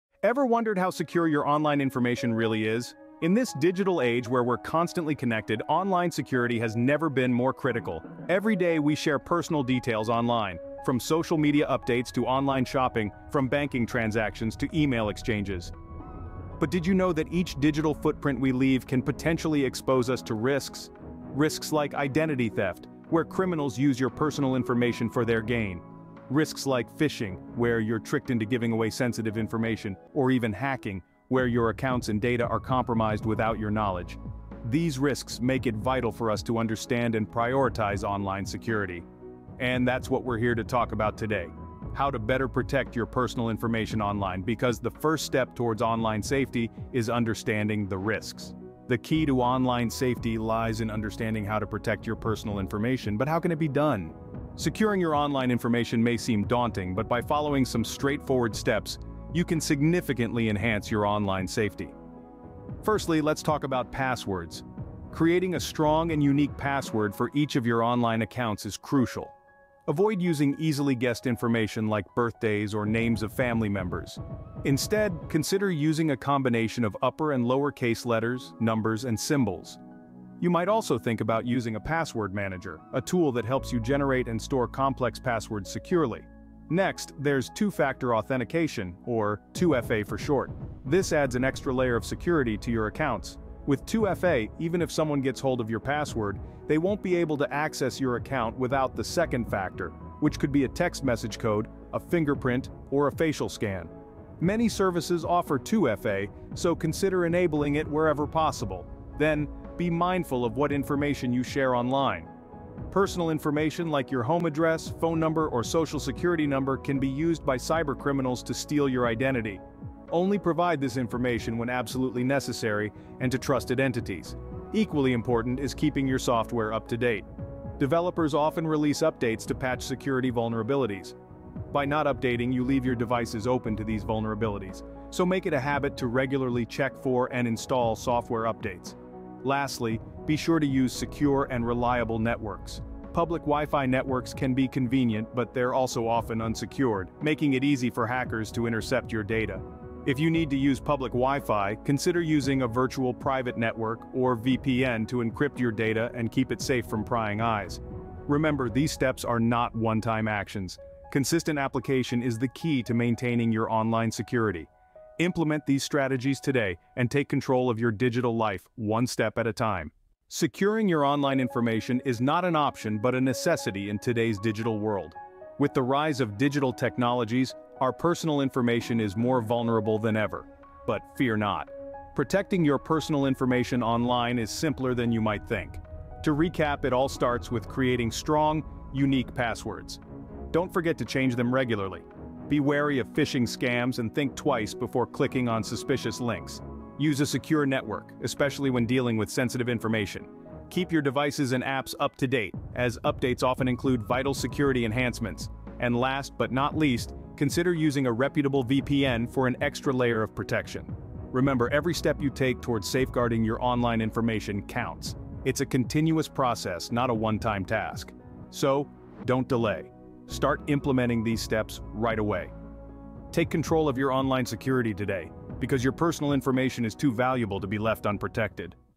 How to Protect Your Personal Information Online
In this digital age, protecting your personal information online has never been more crucial. In our latest video, we've taken it upon ourselves to educate you on the best practices for safeguarding your digital identity. Join us as we provide easy-to-follow steps and practical tips to keep your personal information secure from prying eyes.

From setting up strong passwords to recognizing phishing attempts, we'll cover it all. Our expert advice and real-life examples will empower you to take control of your online privacy and ensure a safer browsing experience for yourself and your loved ones.

But it doesn't stop there! We have an important call to action for you. By implementing these measures and spreading the word, you can help create a more secure cyber environment for everyone. Together, we can make a difference and protect ourselves against the ever-present threats of the digital world.

Watch now and learn how to fortify your digital fortress!

OUTLINE:

00:00:00 The Importance of Online Security
00:00:54 Easy Steps to Protect Your Online Information
00:02:50 Summary and Call to Action